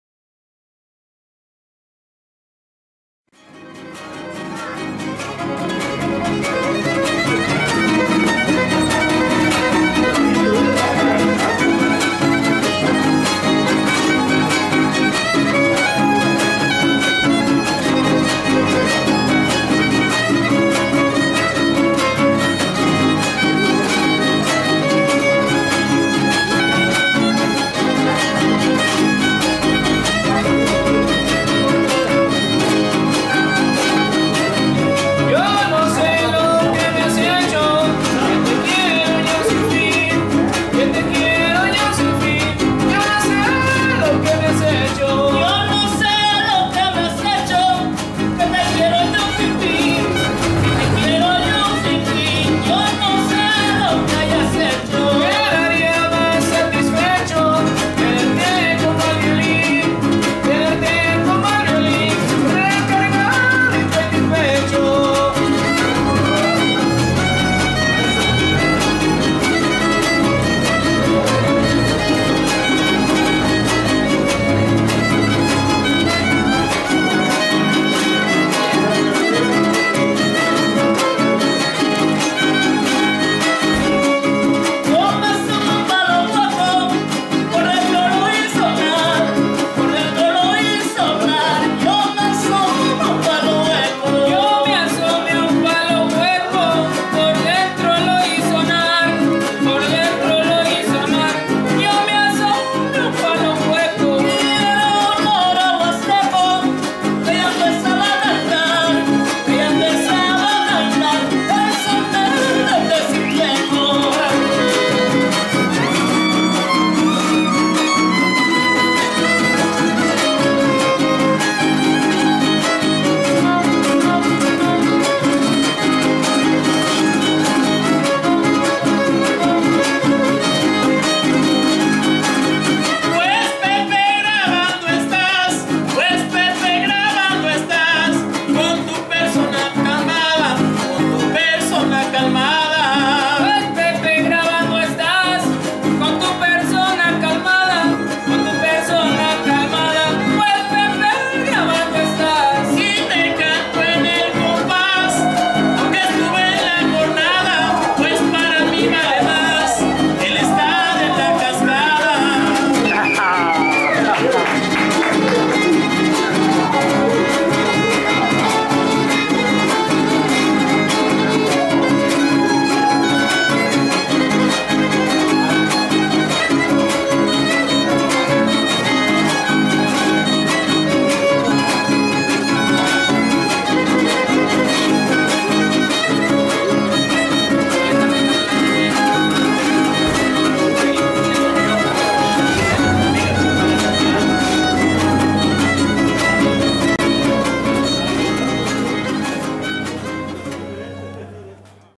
Colatlán. Convivencia en la Cascada del Huapango
Como siempre, las convivencias abajo del escenario nutren de sapiencia y alegría a los asistentes que participamos pra la concurrencia.
Fuera de tensiones y prisas porque se llega el turno y los que siguen no están, por ejemplo, se sigue con la fiesta o empieza antes de subir a escena.